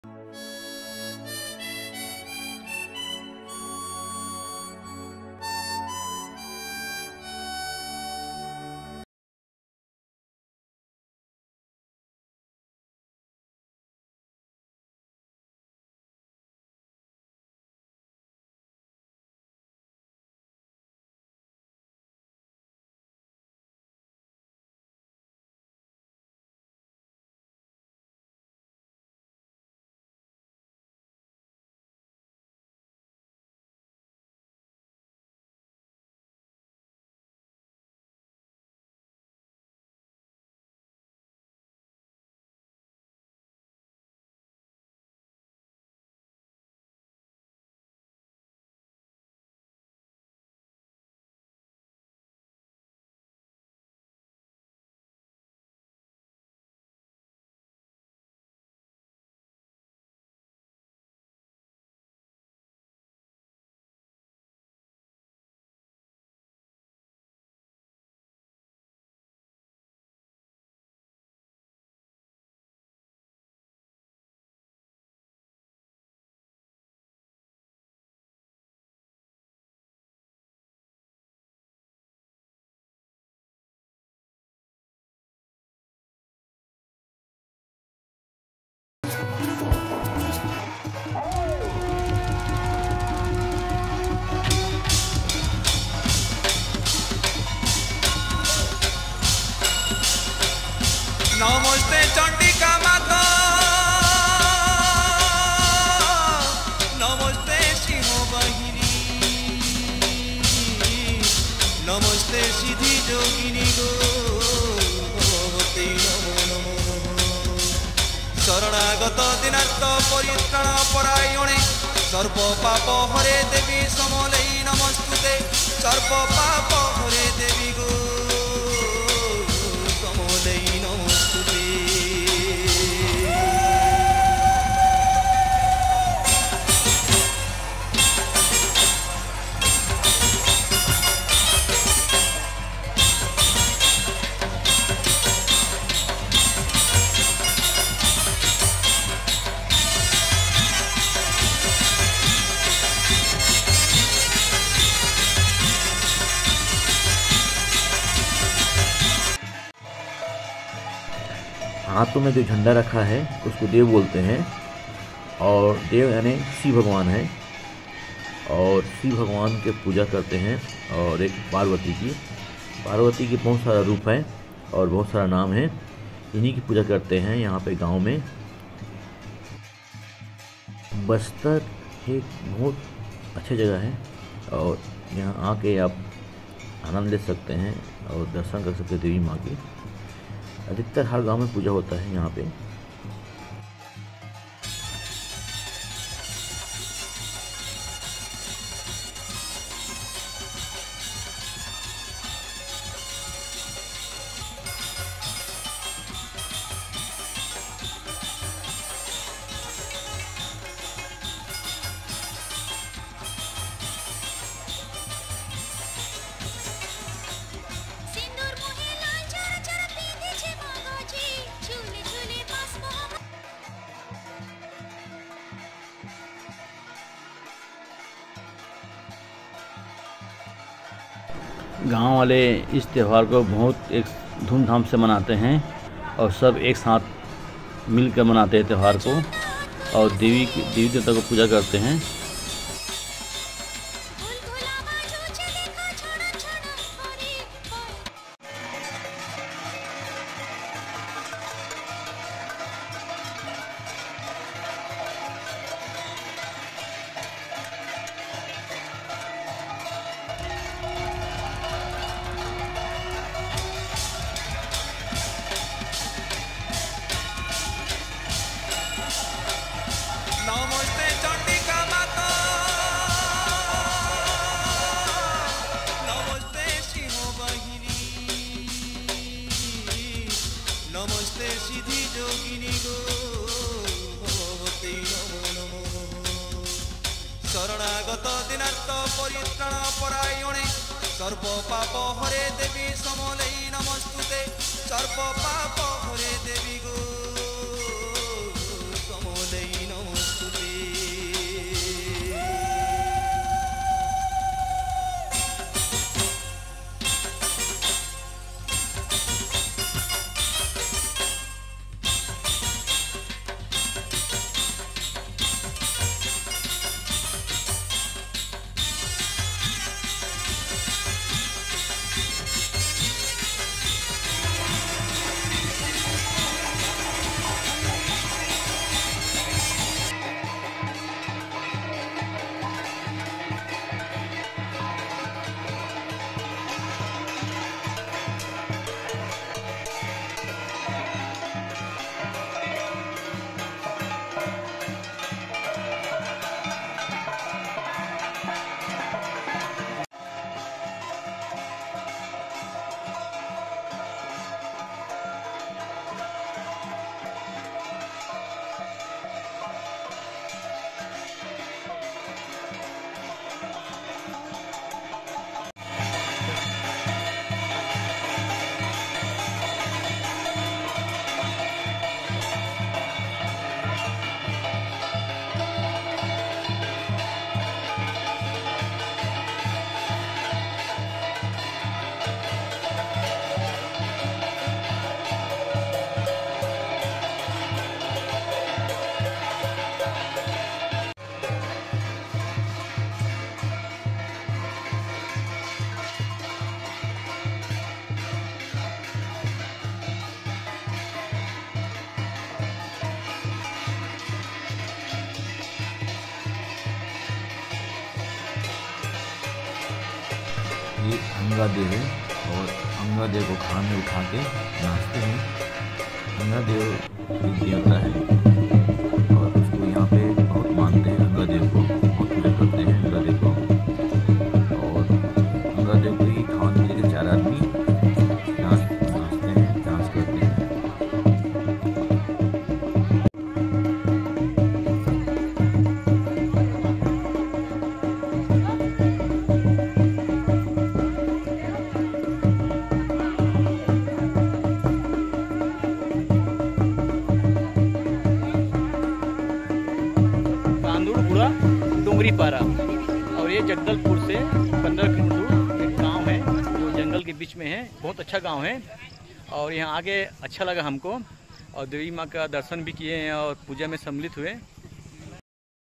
आदिवासी परम्परा मैं पूजा त्योहार👹🛕 |Tribal village Kuldevta Puja |Safar Hai Suhaana Vlog
jagdalpur (chhattisgarh)se 15 km dur village udarguda ka
Devi maa puja kiya gaya
bahut shanti se huwa ...
bastar me har village me gudi parv festival manaya jata hai

mela aur khane pine ka dukan laga rahta hai bahut achha lagta hai

kare Safar Hai Suhaana vlog

आदिवासी परम्परा मैं पूजा त्योहार  || Tribal | Bastar | tribal dance  ‎@Safar Hai Suhaana Vlog

Safar Hai Suhaana Vlog

सफर हैं सुहाना ब्लॉग